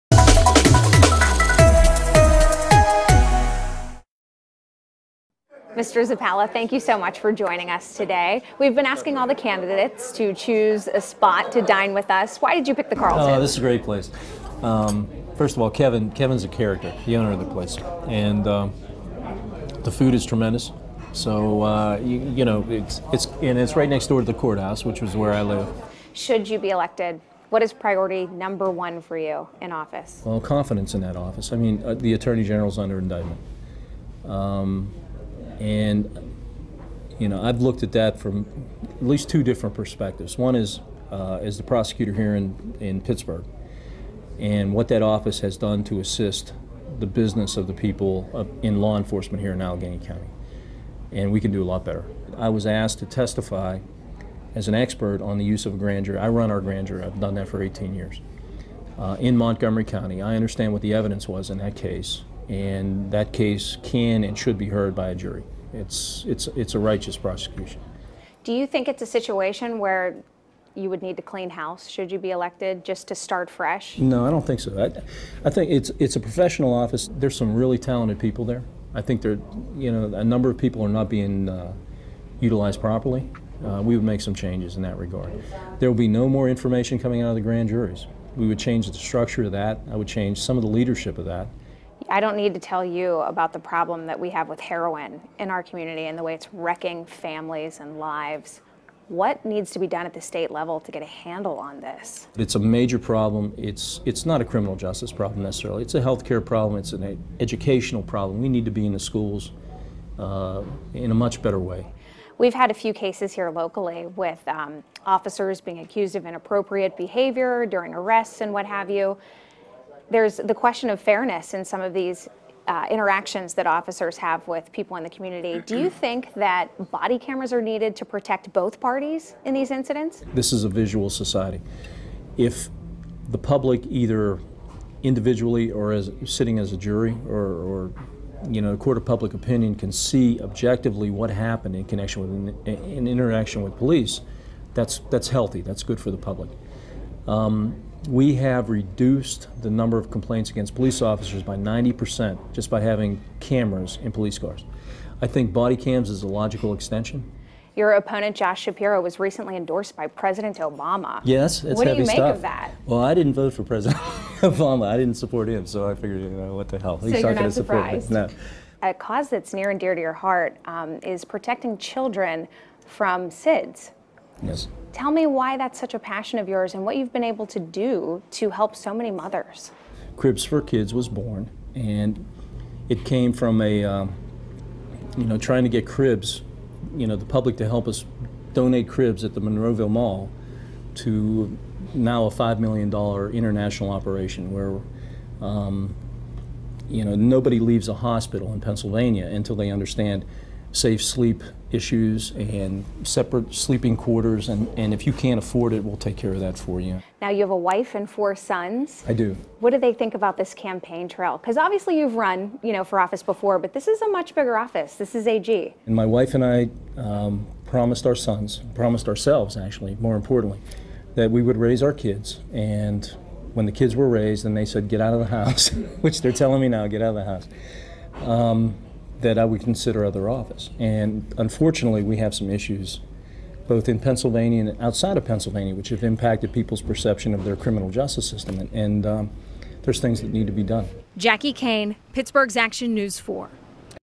Dinner with the Candidates: AG hopeful Stephen Zappala
Pittsburgh's Action News 4 anchor Jackie Cain sat down with Allegheny County District Attorney Stephen Zappala to talk about why he's running to be Pennsylvania's Attorney General.